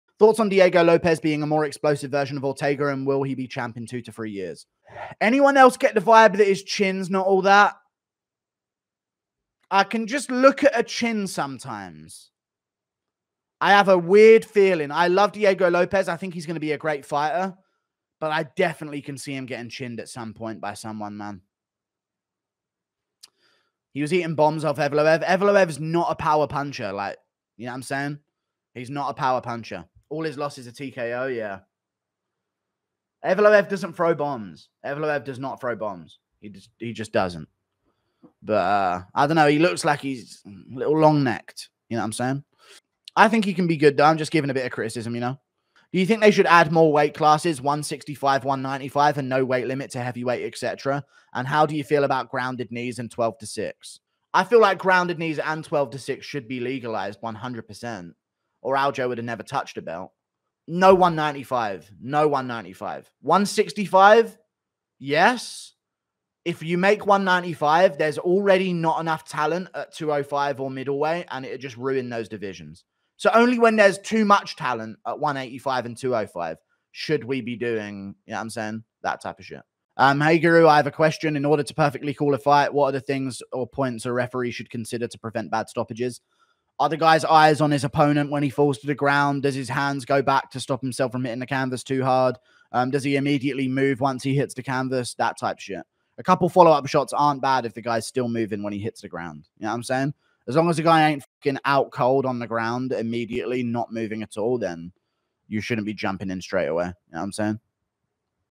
MMA Guru predicts Diego Lopes potential !
MMA Guru predicts Diego Lopes potential and discusses potential UFC rule changes